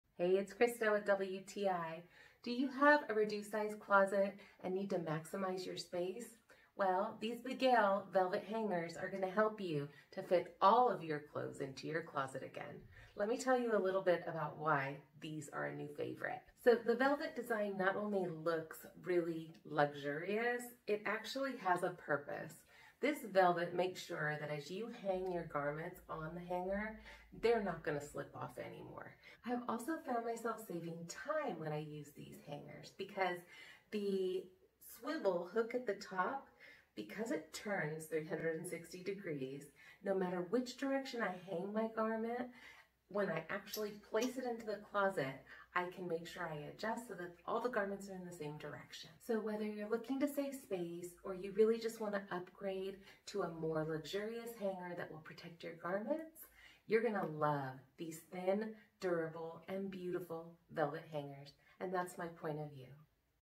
BAGAIL Velvet Hangers | Our Point Of View
Check Our New Website For Amazing Deals!
(Commissionable Links)

PREMIUM QUALITY - Constructed of 80% ABS plastic and 20% velvet flocking,Bagail clothes hangers are sturdy and durable enough for repeated daily use. Comes in a pack of 50 and a standard size of 17.72 inchx9.06 inch Bagail clothing hangers are suitable for hanging most light clothes like pants,blouse,shirt,pajamas,skirts,etc.heavy winter clothing are not recommended for this hanger.
NON SLIP NOTCHES AND FLOCKED VELVET - Equipped with 2 shoulder notches on both sides and covered with luxurious flocked velvet,Bagail clothes hangers grips delicate items such as camisoles and tanks shirts, skirts, dresses and pants with ease to prevent them from slipping off.
PROTECT YOUR SUITS' SHAPE - Reinforced rounded edge provides better support to preserve the shape of your garment.The soft surface could protect your clothes from creases or marks.Graceful silhouette will add some modern aesthetic to your home,dorm,office or laundry room.
WARM NOTICE - Hanging wet clothes are not recommended as it may cause the velvet coating to depreciate.If velvet coating sheds or leaves lint in the dryer, you can use a multi-handle lint brush to clean it. If you’re really worried about the velvet rubbing off,try another color instead of black.
1 YEAR WARRANTY - We stand behind our product and will try our best to make you totally satisfied with your purchase.If you are not pleased with our product,please let us know and we will help you with your order.